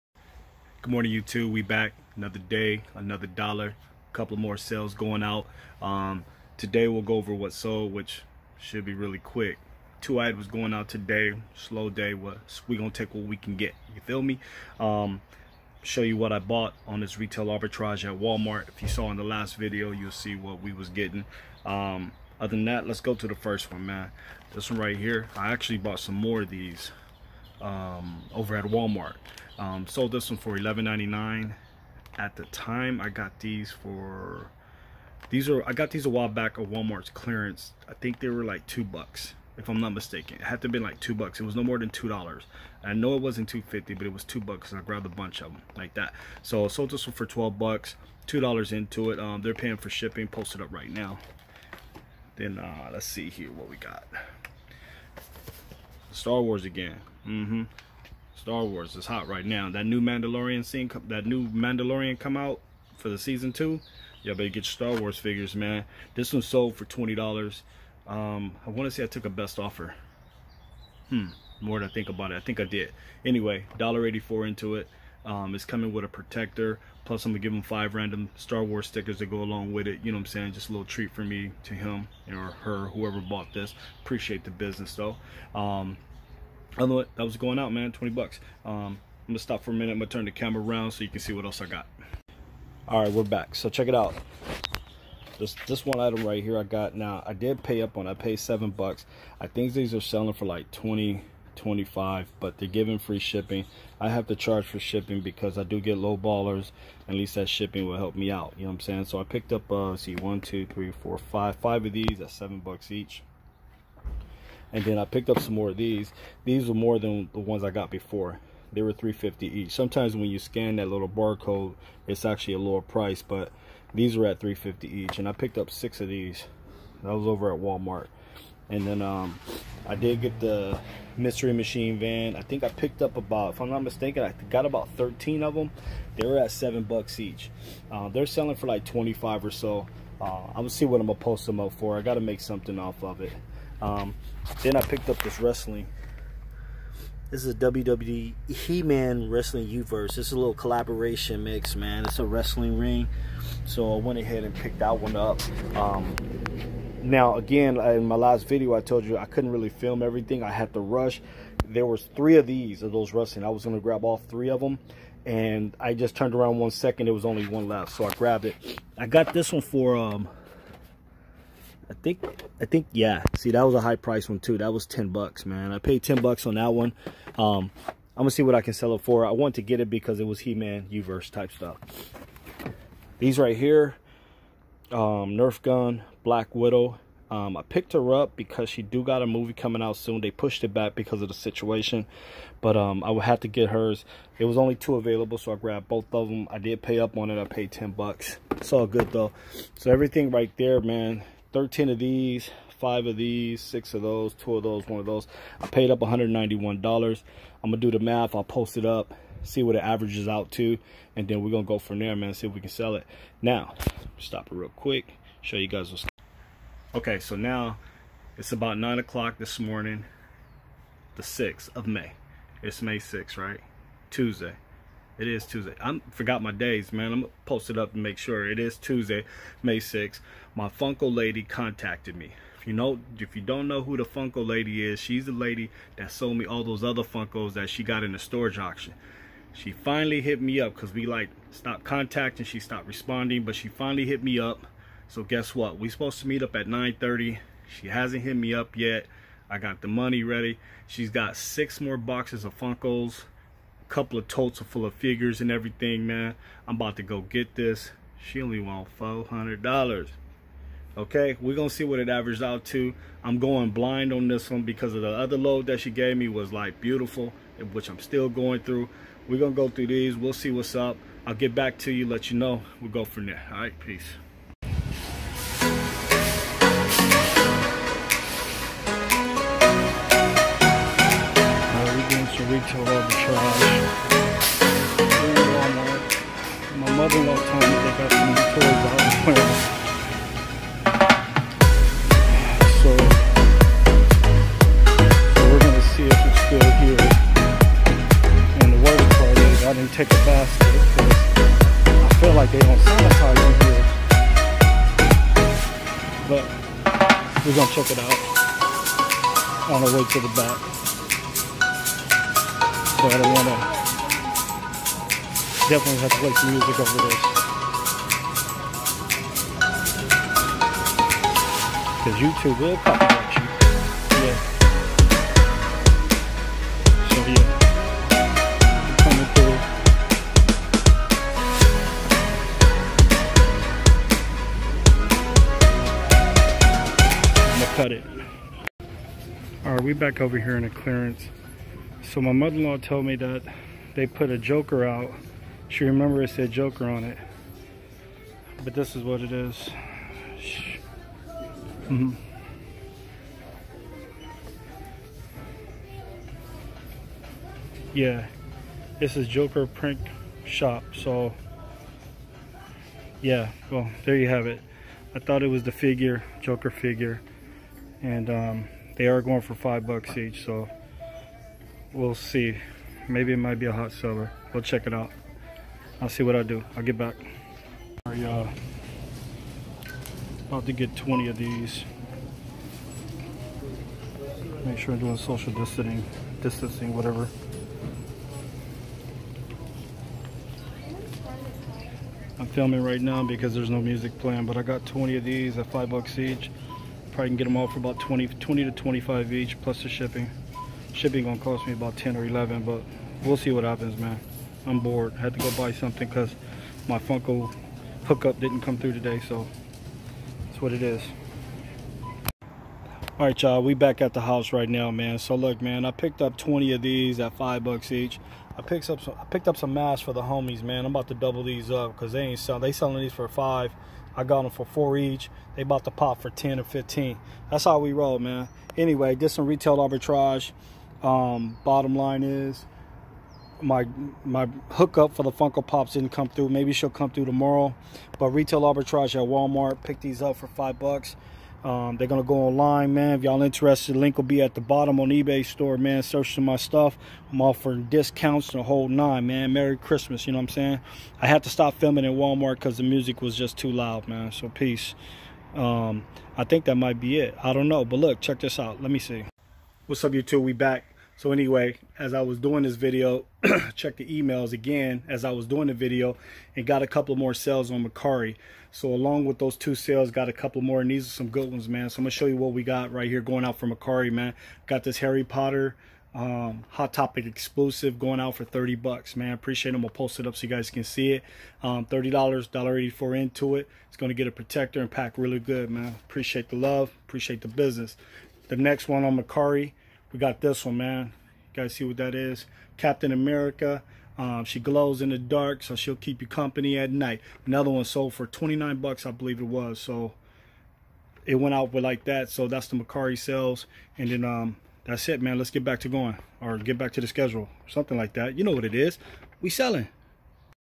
Just a reseller doing some retail Arbitrage.
In this video we do what sold and some retail arbitrage, and some late sales that came in.
Make sure you check out our Ebay store for some great deals...

Ebay Store: